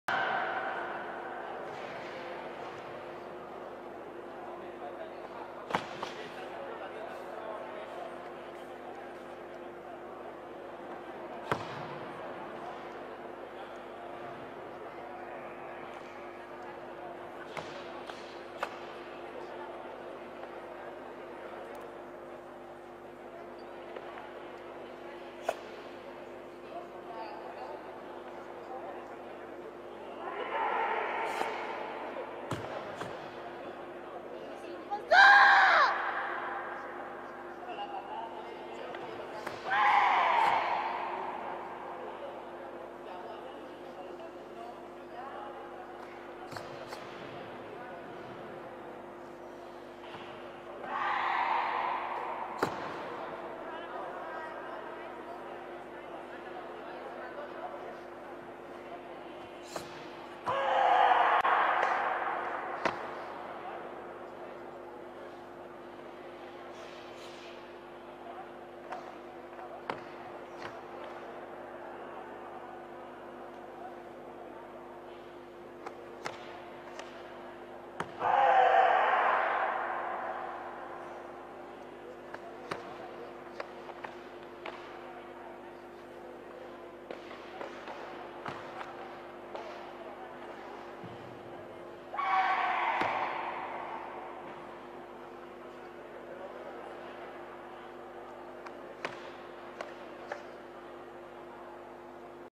Sandra Sanchez (ESP) - Kururunfa - Spanish National Championships 2021
The current female kata world champion performing goju-ryu kata Kururunfa at the Spanish National Championships qualifier rounds 2021.

She won the competition.